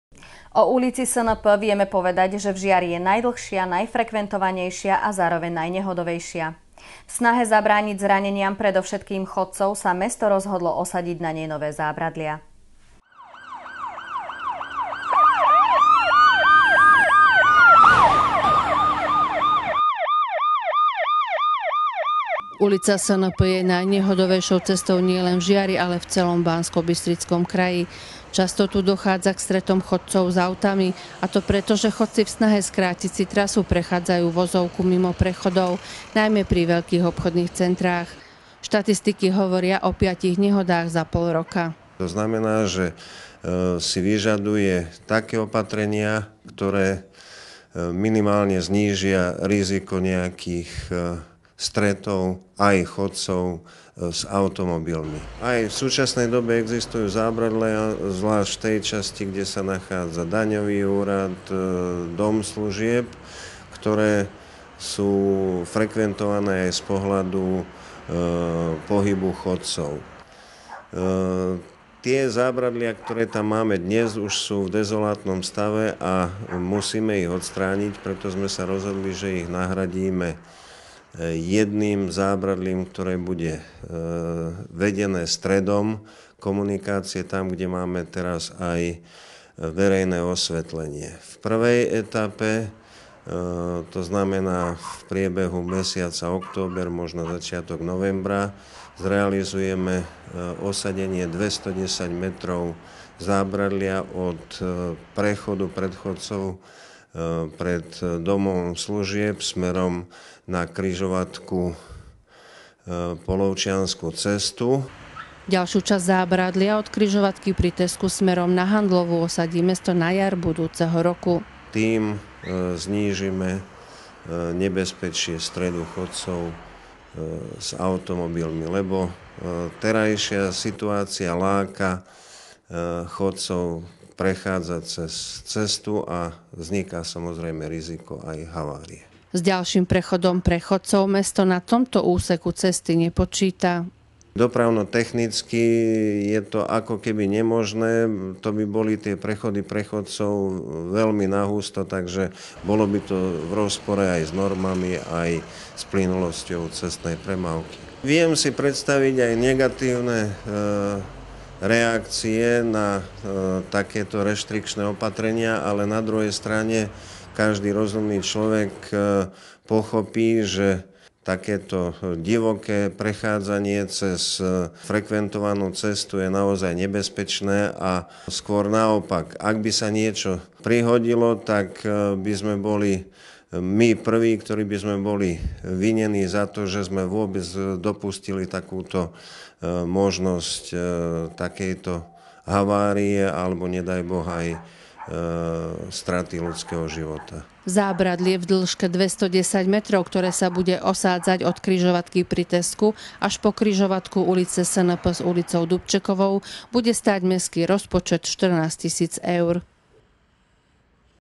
Na ulici SNP pribudnú zábradlia
O ulici SNP vieme povedať, že v Žiari je najdlhšia, najfrekventovanejšia a zároveň najnehodovejšia. V snahe zabrániť zraneniam predovšetkým chodcov sa mesto rozhodlo osadiť na nej nové zábradlia.